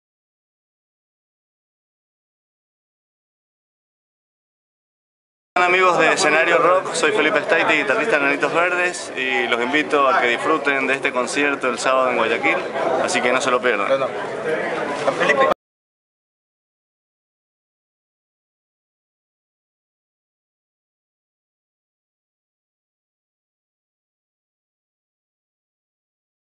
Enanitos Verdes Invitacion de Felipe guitarrista a Escenario Rock
Invitación a la gente de Escenario Rock